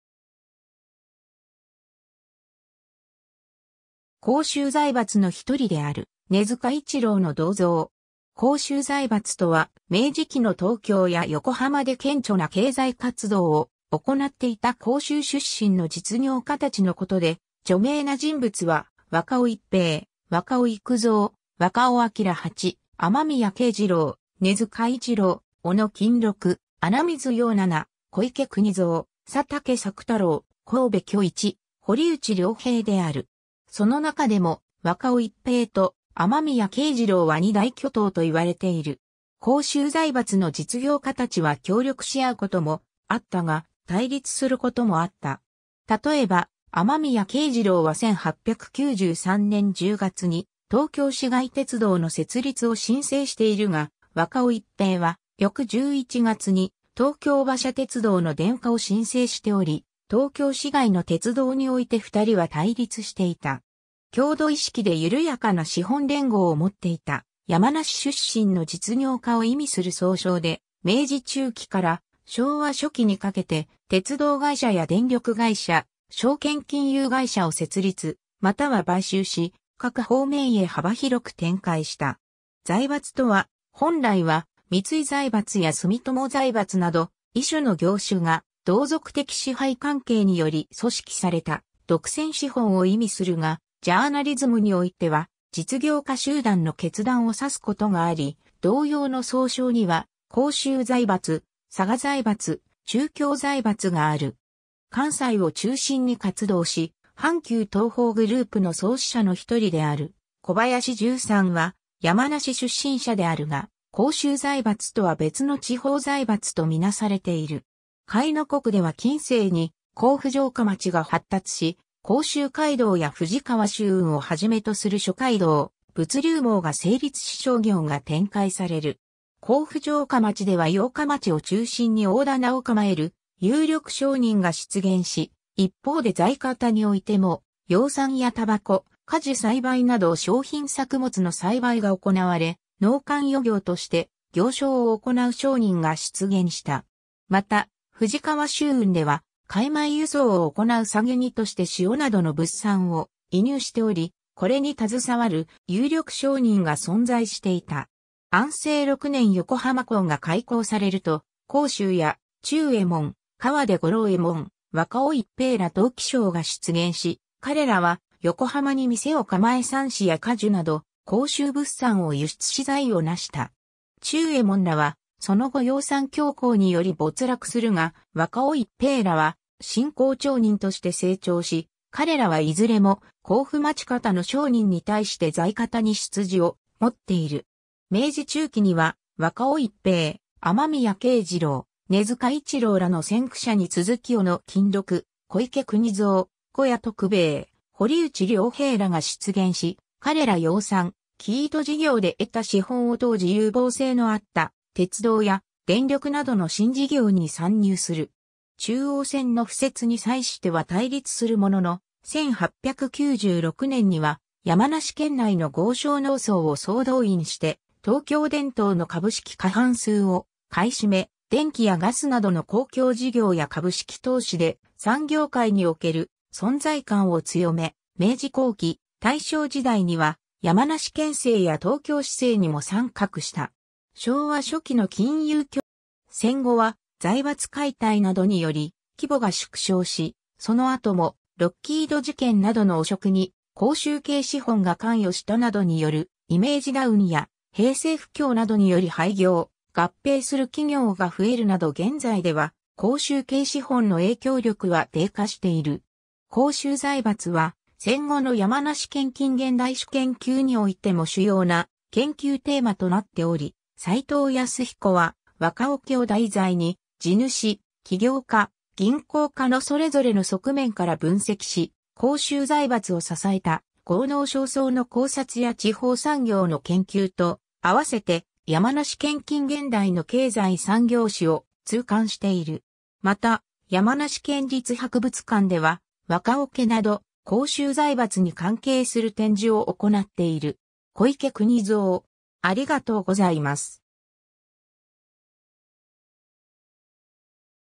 甲州財閥
日本の財閥・コンツェルン
甲州財閥の一人である根津嘉一郎の銅像。
 甲州財閥（こうしゅうざいばつ）とは、明治期の東京や横浜で顕著な経済活動を行っていた甲州（山梨県）出身の実業家たちのことで、著名な人物は、若尾逸平、若尾幾造、若尾璋八、雨宮敬次郎、根津嘉一郎、小野金六、穴水要七、小池国三、佐竹作太郎、神戸挙一、堀内良平である。
その中でも若尾逸平と雨宮敬次郎は二大巨頭と言われている。
甲州財閥の実業家たちは協力し合うこともあったが、対立することもあった。
例えば、雨宮敬次郎は1893年10月に東京市街鉄道の設立を申請しているが、若尾逸平は翌11月に東京馬車鉄道の電化を申請しており、東京市街の鉄道（路面電車）において二人は対立していた。
 郷土意識で緩やかな資本連合を持っていた山梨出身の実業家を意味する総称で、明治中期から昭和初期にかけて鉄道会社や電力会社・証券金融会社を設立または買収し、各方面へ幅広く展開した。
 「財閥」とは、本来は三井財閥や住友財閥など異種の業種が同族的支配関係により組織された独占資本を意味するが、ジャーナリズムにおいては実業家集団の結団を指すことがあり、同様の総称には江州財閥（近江商人）、佐賀財閥、中京財閥がある。
 関西を中心に活動し、阪急東宝グループの創始者の一人である小林一三は山梨出身者であるが、甲州財閥とは別の地方財閥と見なされている。
 甲斐国では近世に甲府城下町が発達し、甲州街道や富士川舟運をはじめとする諸街道・物流網が成立し商業が展開される。
甲府城下町（新府中）では八日町（甲府市中央）を中心に大店を構える有力商人が出現し、一方で在方においても養蚕や煙草、果樹栽培など商品作物の栽培が行われ、農間余業として行商を行う商人が出現した。
また、富士川舟運では廻米輸送を行う下げ荷として塩などの物産を移入しており、これに携わる有力商人が存在していた。
 安政6年（1859年）横浜港が開港されると、甲州屋忠右衛門（篠原忠右衛門）、川手五郎右衛門、若尾逸平ら投機商が出現し、彼らは横浜に店を構え蚕糸や果樹など甲州物産を輸出し財をなした。
忠右衛門らはその後養蚕恐慌により没落するが、若尾逸平らは新興町人として成長し、彼らはいずれも甲府町方の商人に対して在方に出自をもっている。
 明治中期には若尾逸平、雨宮敬次郎、根津嘉一郎らの先駆者に続き小野金六、小池国三、古屋徳兵衛、堀内良平らが出現し、彼ら養蚕・生糸事業で得た資本を当時有望性のあった鉄道や電力などの新事業に参入する。
中央線の敷設に際しては対立するものの、1896年（明治29年）には山梨県内の豪商農層を総動員して東京電燈の株式過半数を買い占め、電気やガスなどの公共事業や株式投資で産業界における存在感を強め、明治後期・大正時代には山梨県政や東京市政にも参画した。
 昭和初期の金融恐慌で総帥的立場にあった若尾家が没落し、世代交代により郷土意識が希薄化すると影響力は弱体化する。
戦後は財閥解体などにより規模が縮小し、その後もロッキード事件などの汚職に甲州系資本が関与したなどによるイメージダウンや、平成不況などにより廃業・合併する企業が増えるなど現在では甲州系資本の影響力は低下している。
 甲州財閥は戦後の山梨県近現代史研究においても主要な研究テーマとなっており、斎藤康彦は若尾家を題材に地主、企業家、銀行家のそれぞれの側面から分析し、甲州財閥を支えた豪農商層の考察や地方産業の研究と合わせて山梨県近現代の経済産業史を通観している。
また、山梨県立博物館では若尾家など甲州財閥に関係する展示を行っている。
 小池国三 （著者・編者の五十音順）